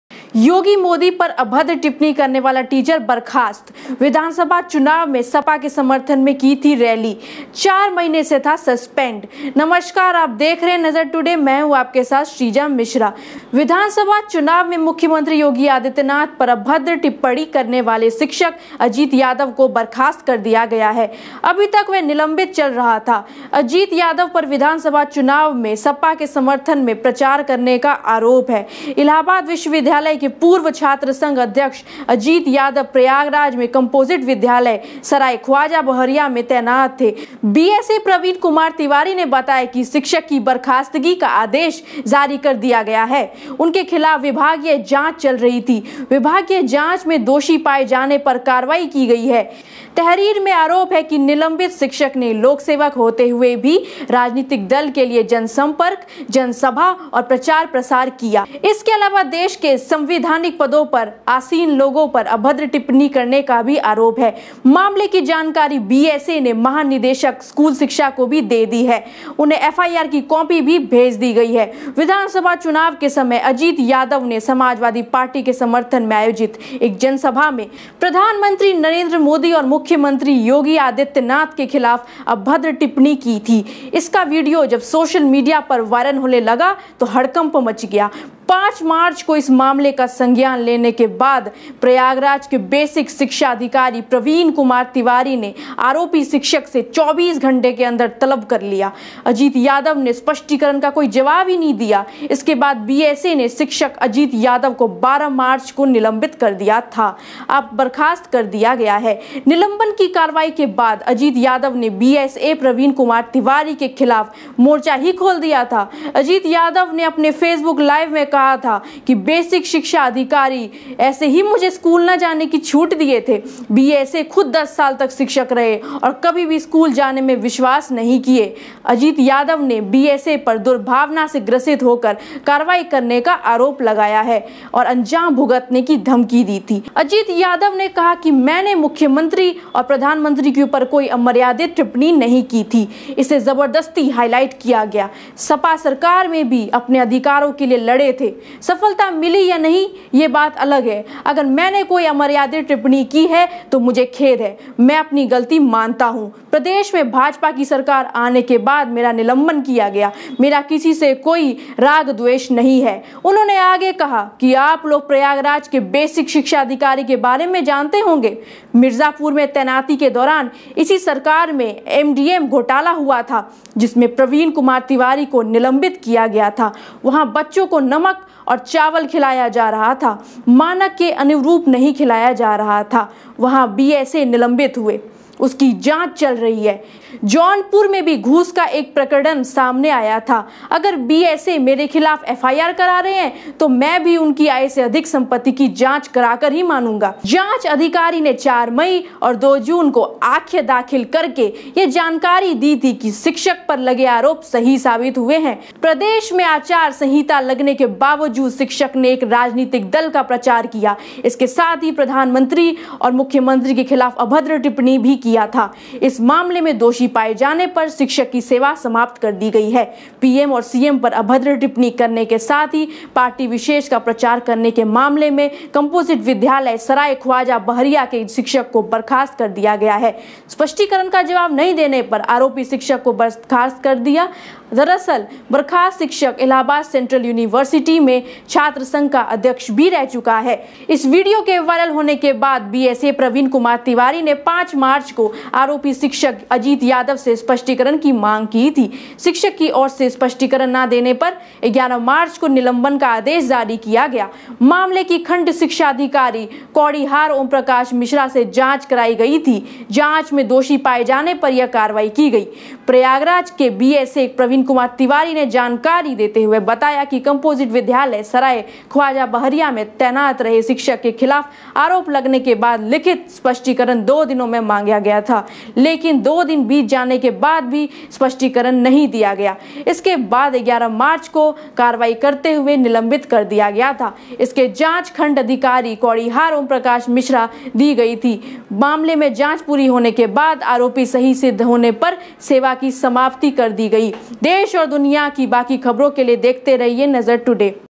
योगी-मोदी पर अभद्र टिप्पणी करने वाला टीचर बर्खास्त
विधानसभा चुनाव में मुख्यमंत्री योगी आदित्यनाथ पर अभद्र टिप्पणी करने वाले शिक्षक अजीत यादव को बर्खास्त कर दिया गया है। अभी तक वह निलंबित चल रहा था। अजीत यादव पर विधानसभा चुनाव में सपा के समर्थन प्रचार करने का आरोप है। इलाहाबाद विश्वविद्यालय के पूर्व छात्रसंघ अध्यक्ष अजीत यादव प्रयागराज में कंपोजिट विद्यालय सराय ख्वाजा बहरिया में तैनात थे।